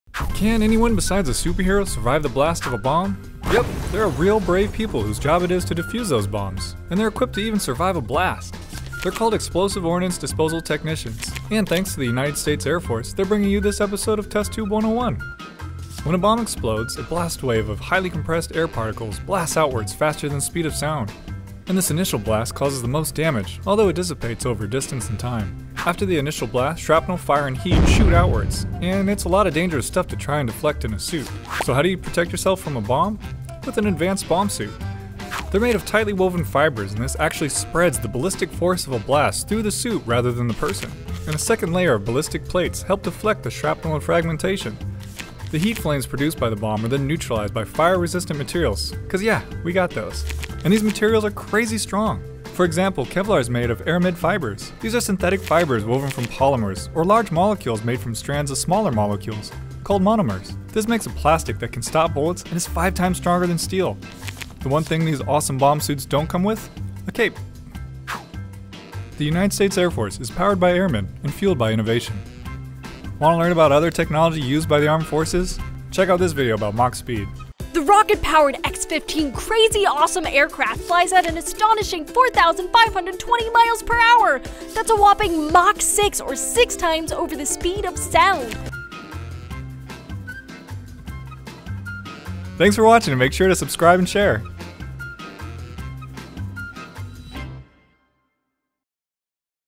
How Does A Bomb Suit Protect You From A Bomb?
So much can go wrong when you're trying to defuse a bomb. Thanks to bomb suits a lot of those factors are negated, but how?